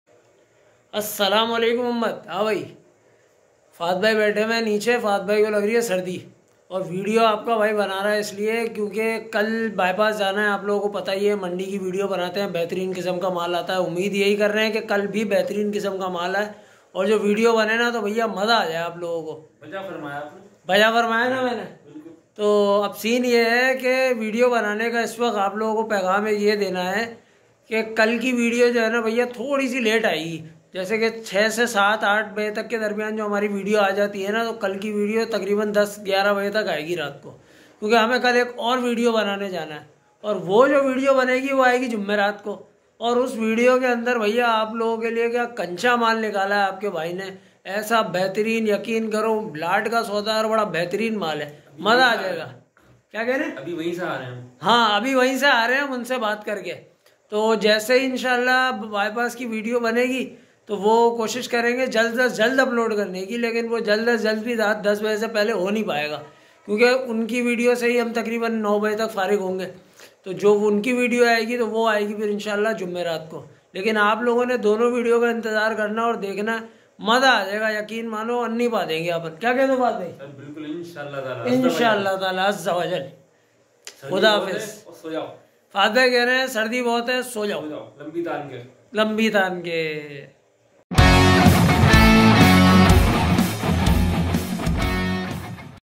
Hyderebad Bypass Mandi K Hawale Se Update | Cow Mandi 2023 | Ramzanis
Talk about goat and cattle market price update | Pets and Animals